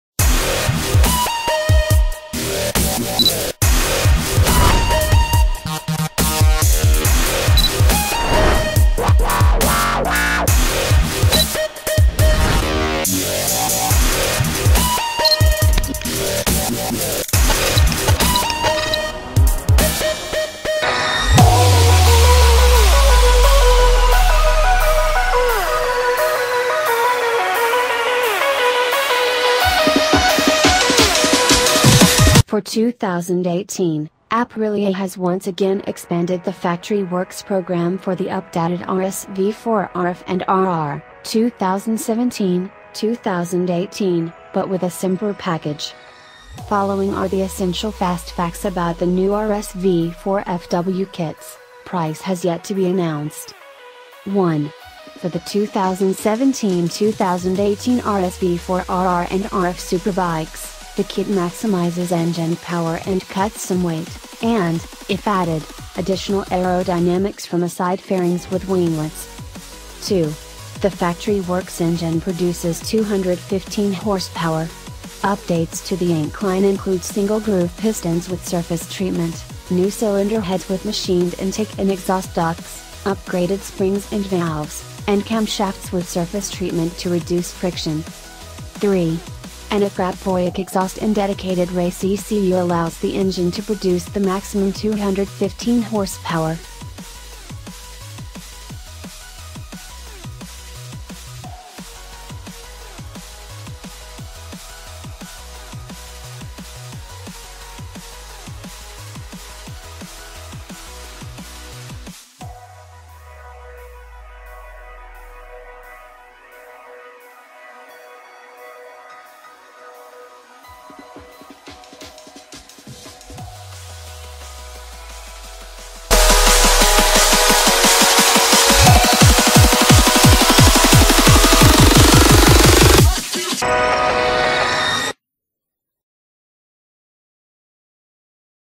LOOK THIS!!!2018 Aprilia RSV4 Factory SE Special Lookaround Price & Spec
LOOK THIS!!!2018 Aprilia RSV4 Factory SE Special Lookaround Price & Spec - In December 2016, Aprilia announced the expansion of its Factory Works (FW) kits for RSV4 platforms.

$16,999